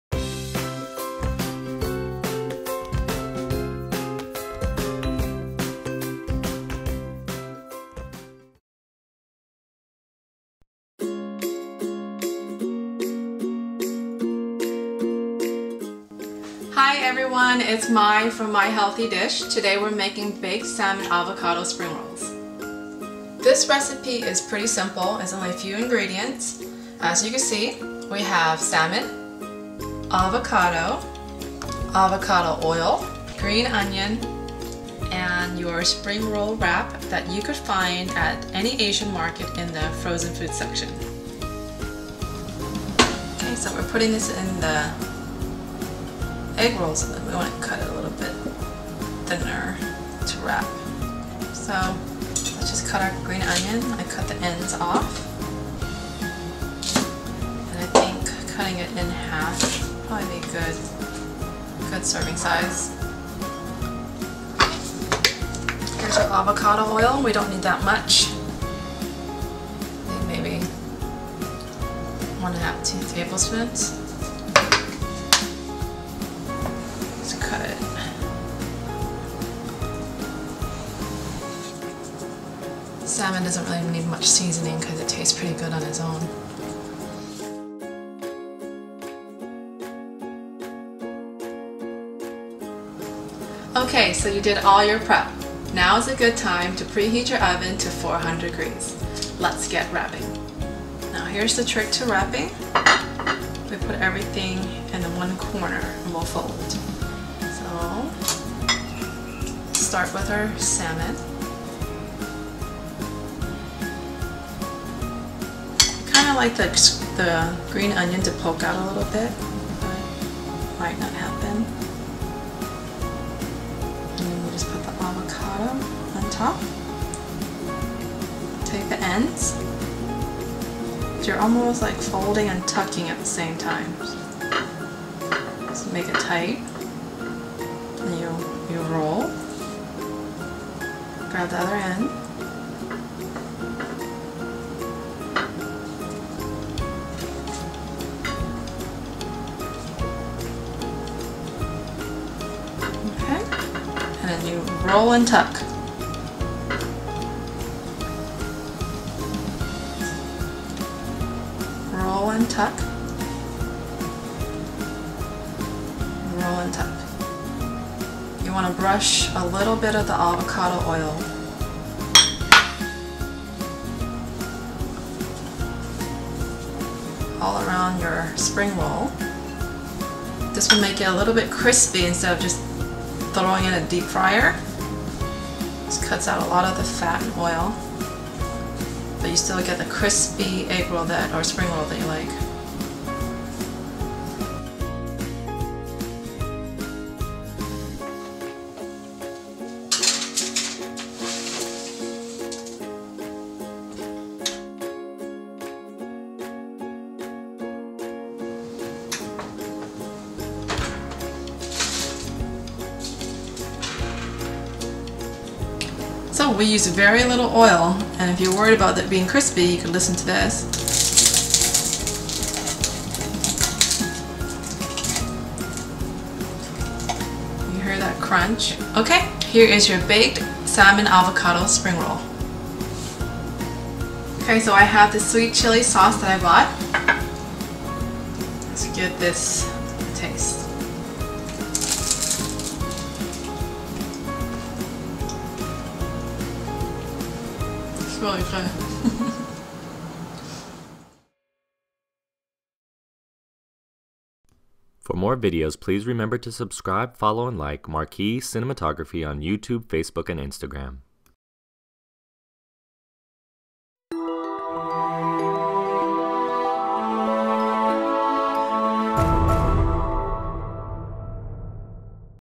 Cooking demo - Baked Salmon Avocado Spring Roll - My Healthy Dish
Cooking demo for Baked Salmon Avocado Spring Roll by My Healthy Dish. Video production by Marquee Cinematography.